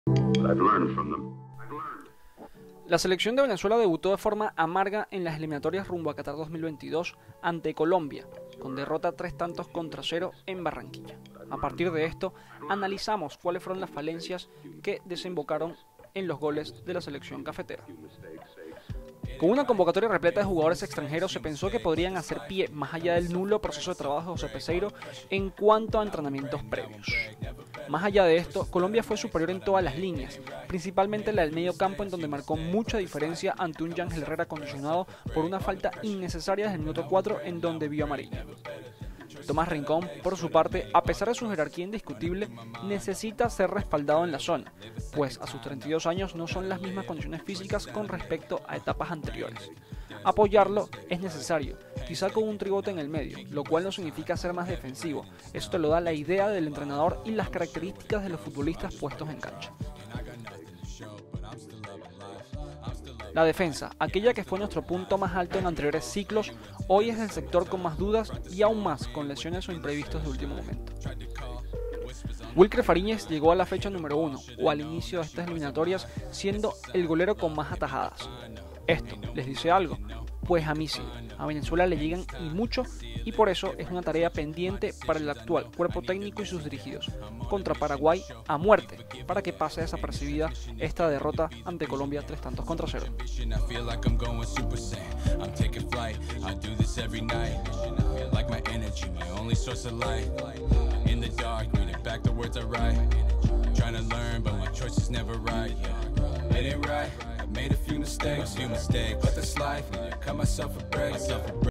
Colombia 3-0 Venezuela - Análisis táctico
Jornada 1 de las Eliminatorias rumbo a Qatar 2022.

9 de octubre 2020, Barranquilla.

Análisis del compromiso que terminó 3-0 a favor del cuadro cafetero.